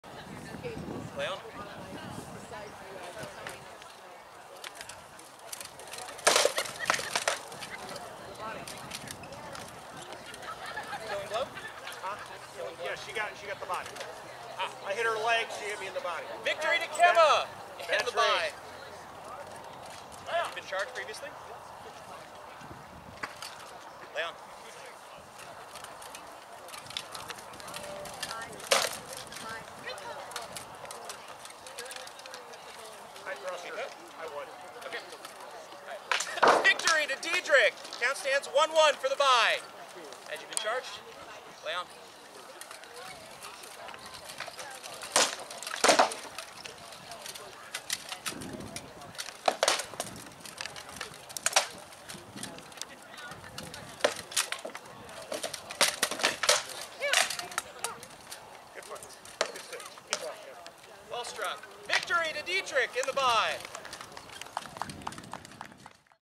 Fall Crown 2024, Bout 03 - Dyderich v Kemma (BYE)
Mid-Realm Fall Crown 2024
Captain Dyderich Wolfhart v Lady Kemma of the Three Rivers (BYE)
Recorded in the Barony of Carraig Ban.